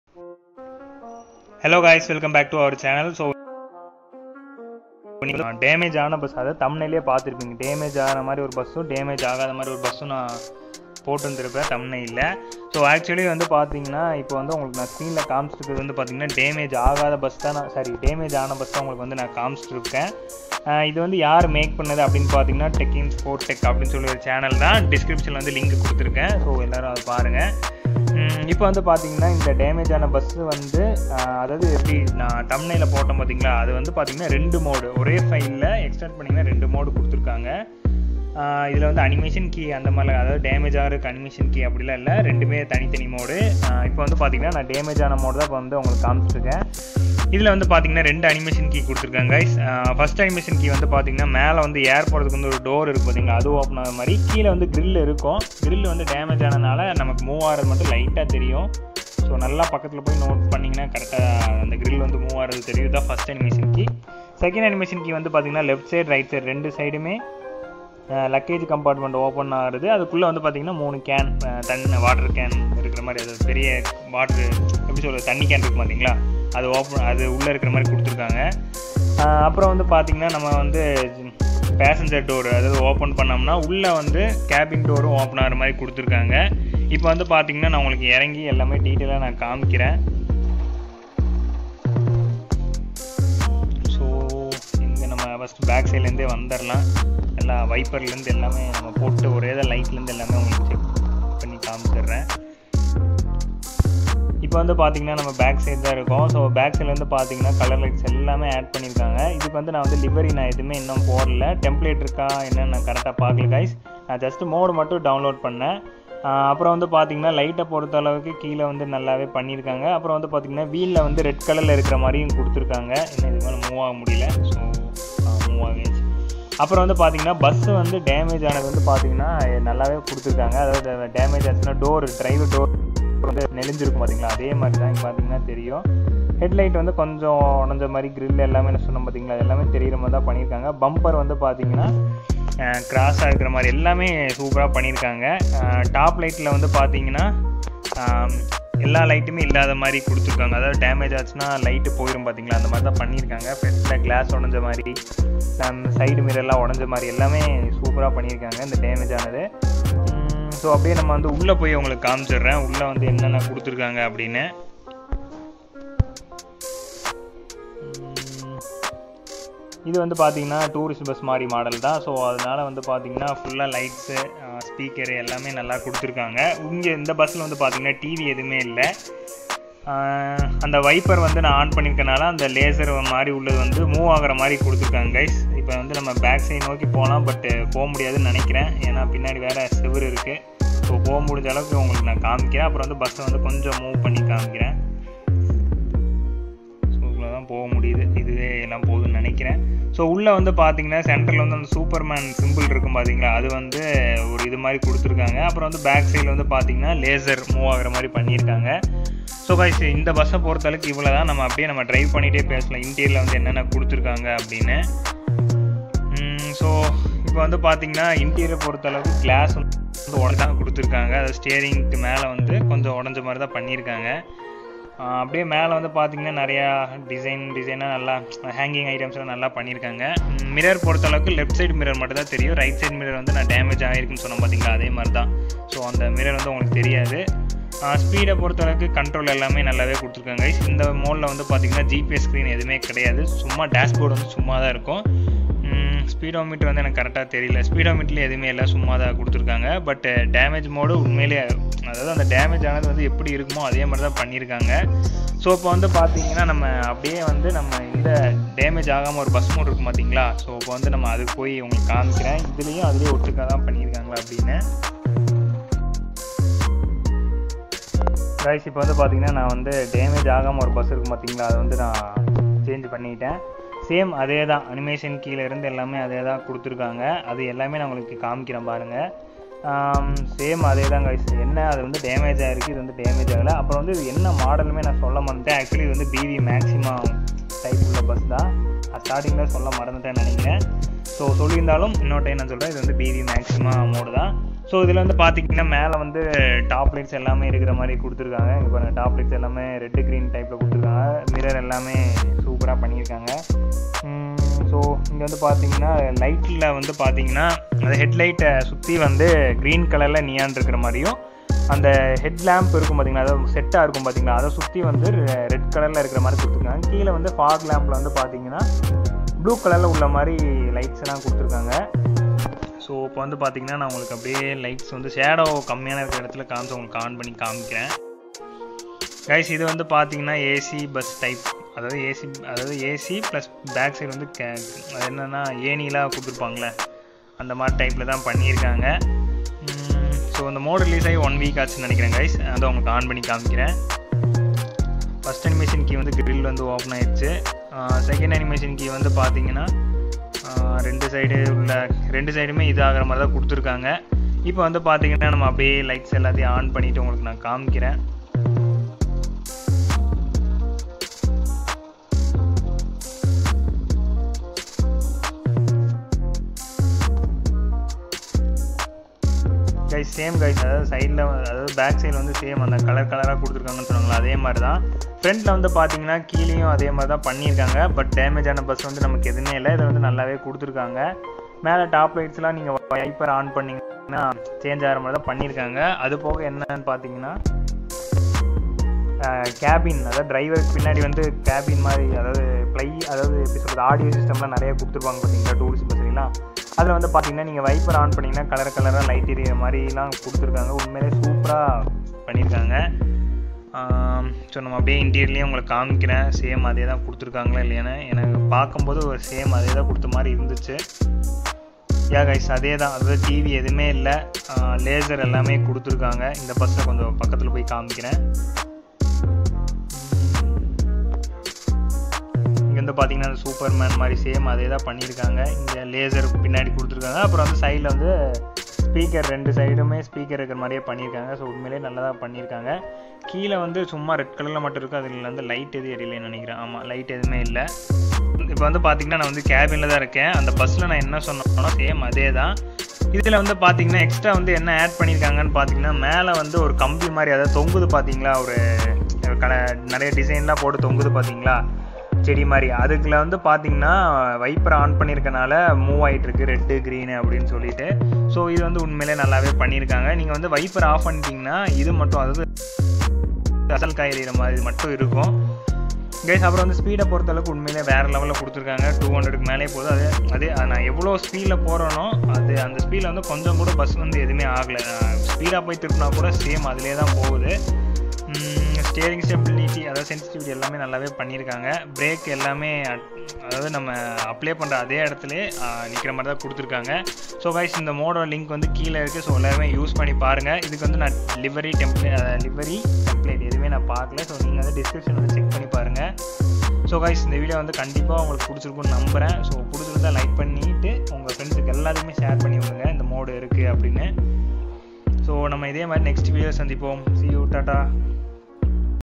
Maxima Crashed Bus Mod & Normal Bus Mod Released | Mod Review | Team ThadikaraN
Hi Guy's … 😃

⚠️ Note : It's Android Game … Use earphones for better Experience ⚠️
Hit bell button 🔔

DON'T REUPLOD OUR VIDEOS 🙏

⚒️mods:
📍Vehicle: bv Maxima bus Mod ( Techies 4 Tech )
📍Editing App: Insort
📍Screen Recorder: ADV Screenrecorder

Drive Safe 😇 Happy Journey